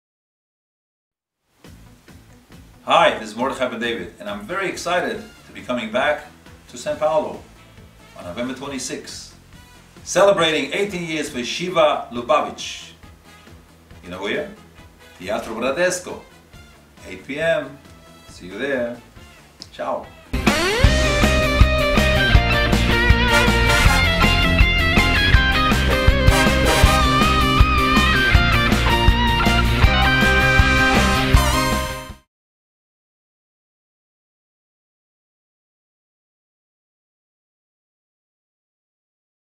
MDB Promo Show Brazil
Promo do Show - Mordechai Ben David & Yossi Green que será realizado pela Yeshiva Lubavitch no dia 26 de novembro de 2012.